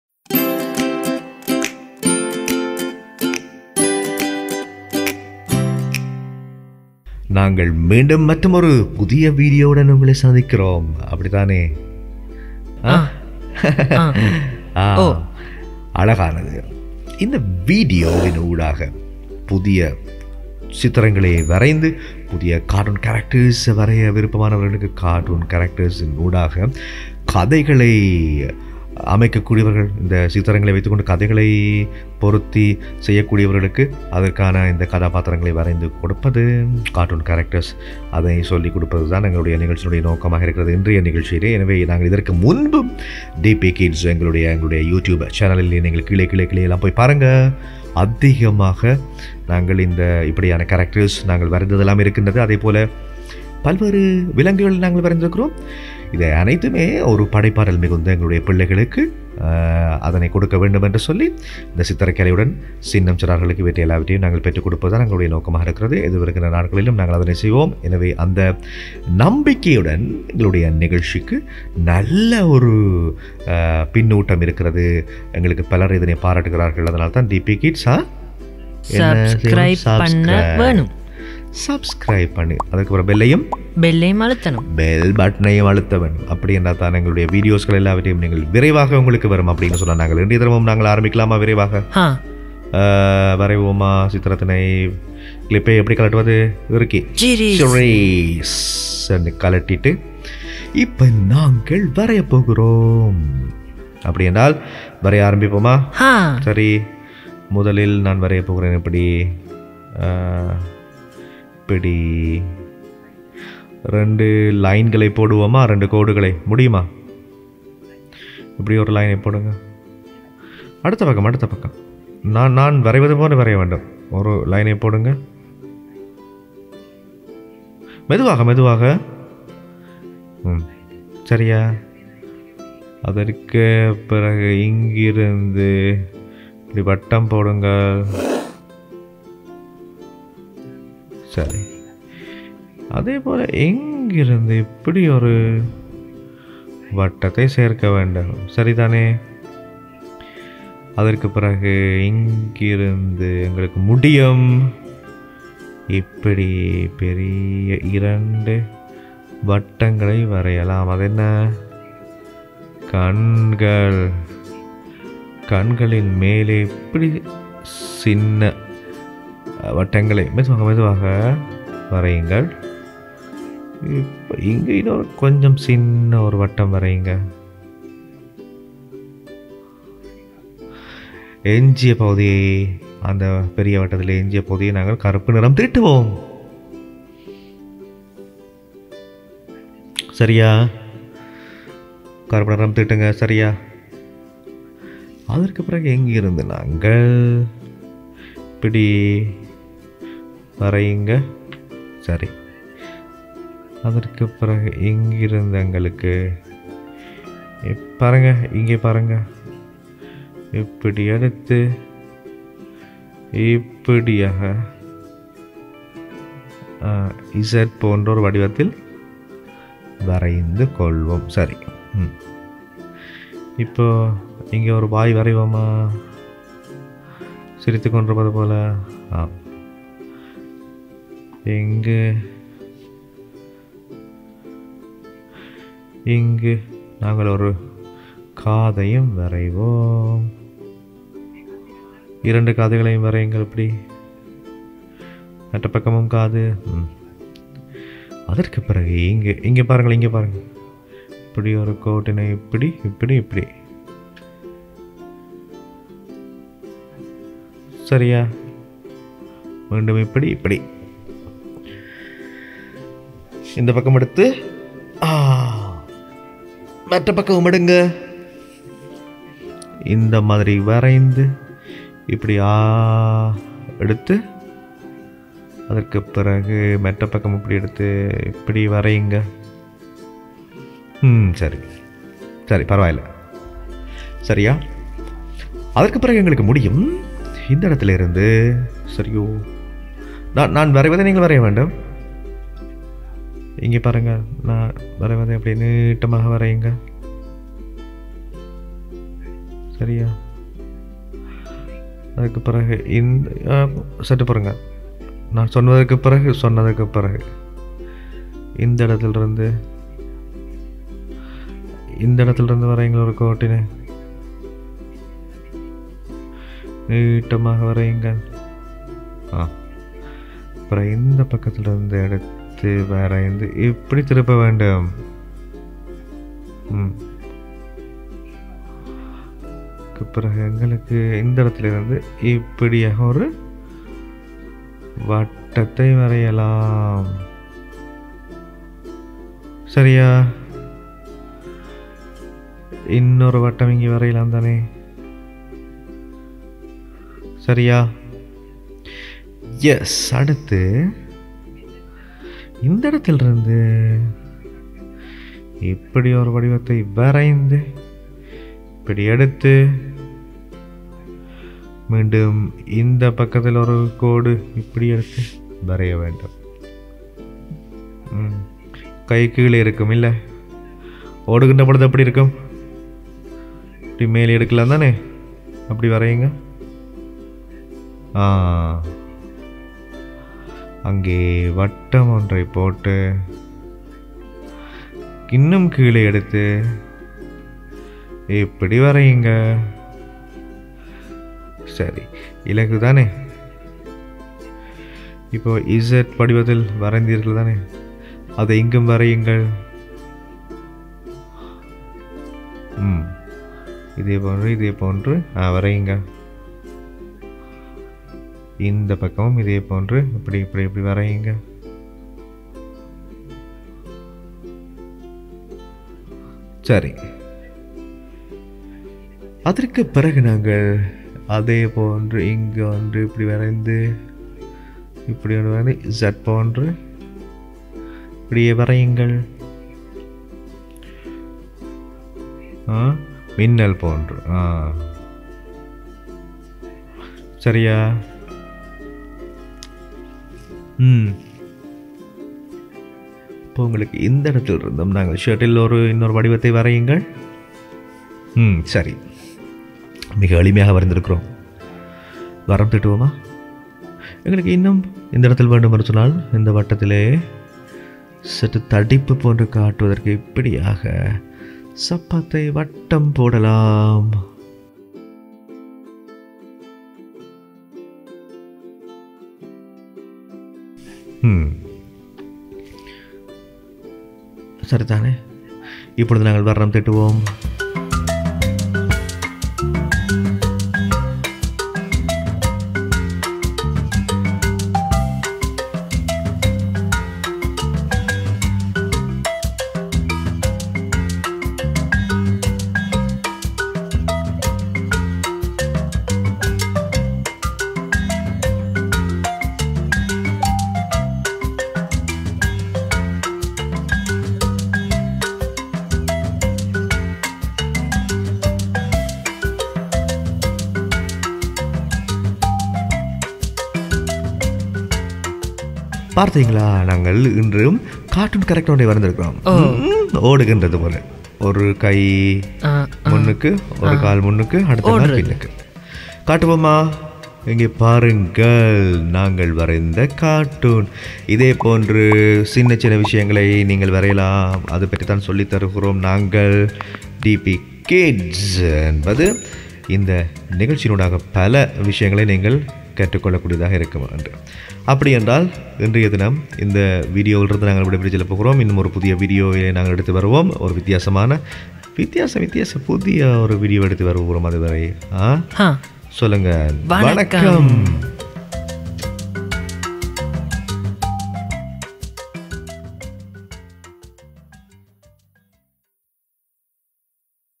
ஒரு கார்ட்டூன் கதாபாத்திரத்தை வரையலாம் - டிபி கிட்ஸ் வரைதல் மற்றும் கைவினை (வீடியோ 83)
▶️ இந்த வீடியோவில் கார்ட்டூன் கதாபாத்திரத்தை எப்படி வரையலாம் என்று பார்ப்போம்
▶️ In this video we will see how to draw a cartoon character
TIMESTAMPS:
அறிமுகம் - 0:00
ஒரு கார்ட்டூன் கதாபாத்திரத்தை வரையலாம்  -  1:56